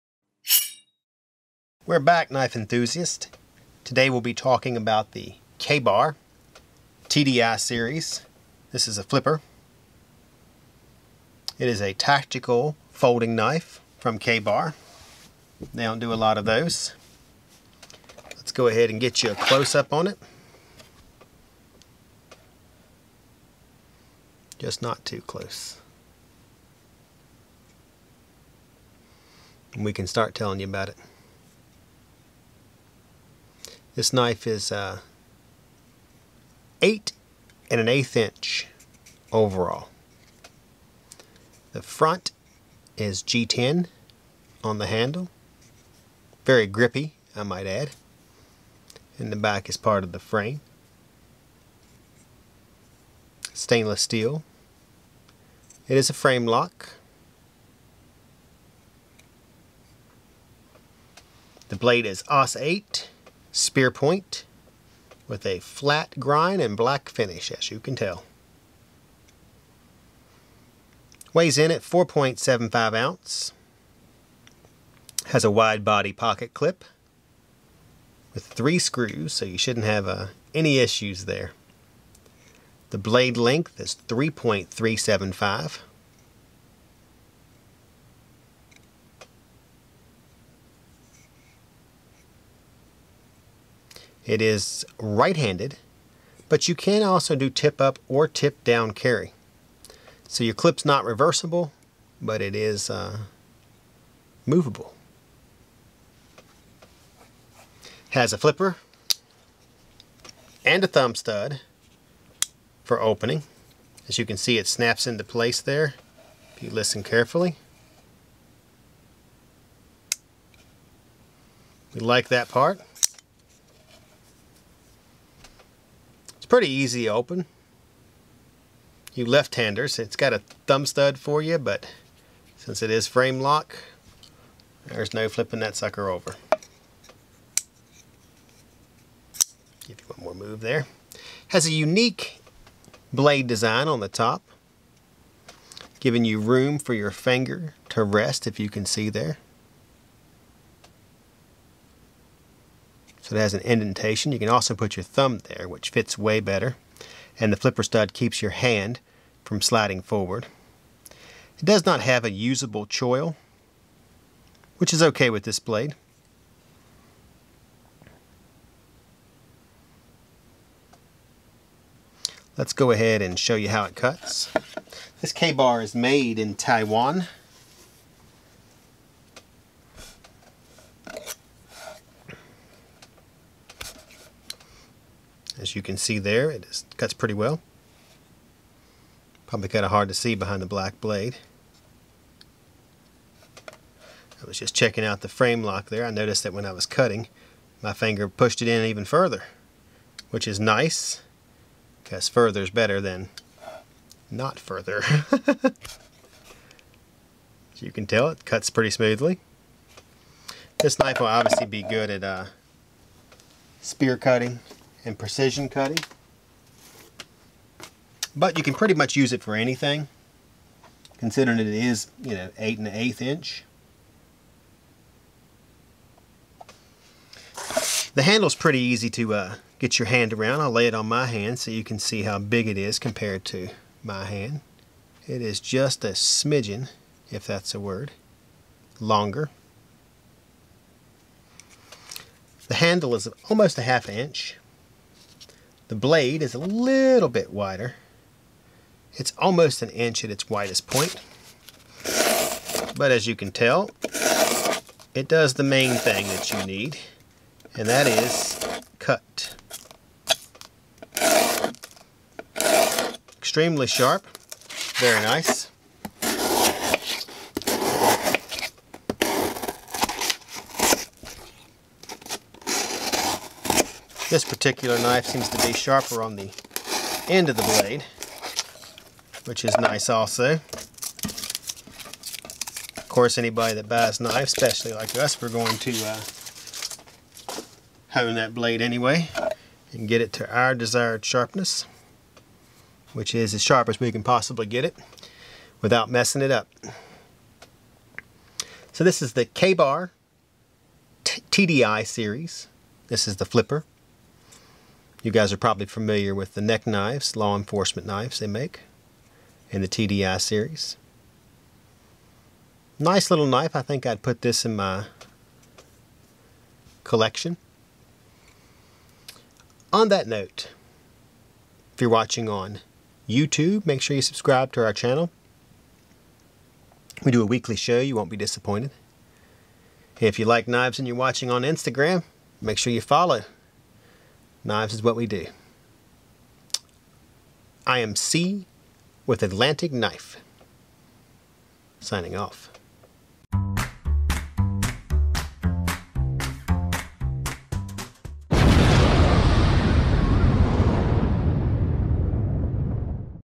Ka-Bar TDI Framelock Folding Knife Review | Atlantic Knife Reviews 2020
Cee with Atlantic Knife reviews the Ka-Bar TDI Framelock folding pocket knife.

Check Out Some Of Our Other Knife Videos Here